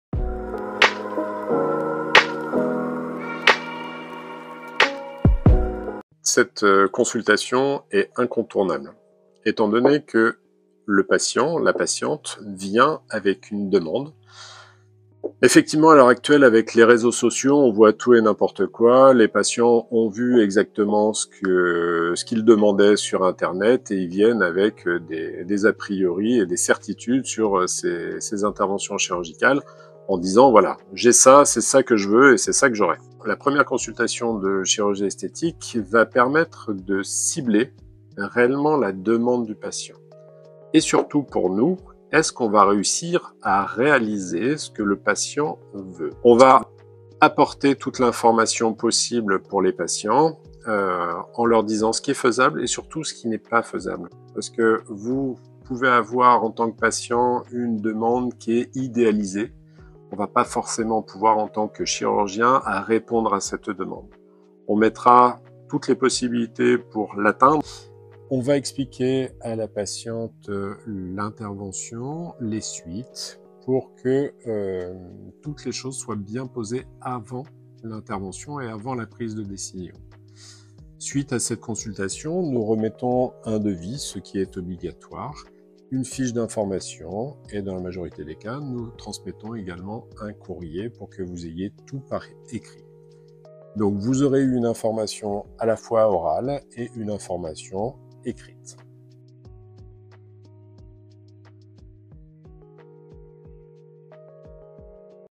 L'importance de la première consultation de chirurgie esthétique - Docteur Frédéric Laurent
La première consultation de chirurgie esthétique est une étape très importante dans le parcours de votre prise en charge à la Clinique Clemenceau.

C’est le premier échange pour établir une relation de confiance avec votre chirurgien. Il sera à l’écoute de vos envies, de vos besoins et parfois de vos complexes que vous souhaitez voir améliorer ou effacer.

Le Docteur Frédéric Laurent est chirurgien plasticien à la Clinique Clemenceau de Lille et d'Arras.

Des questions ? Contactez notre secrétariat au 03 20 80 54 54.